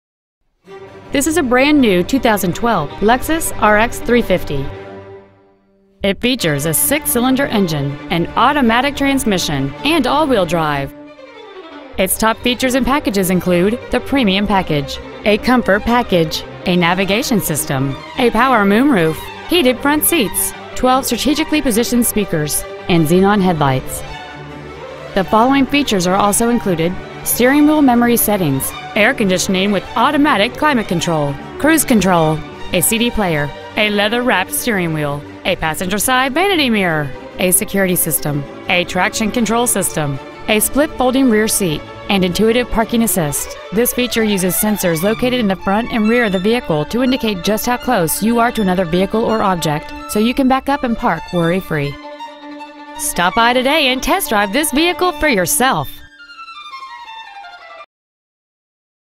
2012 Lexus RX 350 Rockville MD 20855
Year : 2012
 Make : Lexus
 Model : RX 350
 Engine : 3.5L V-6 cyl
 Trans . : 6-Speed Automatic
 Exterior : Satin Cashmere Metallic
 Miles : 3
 Interior : Black

 15501 Frederick Road.

 2012 Lexus RX 350 Rockville MD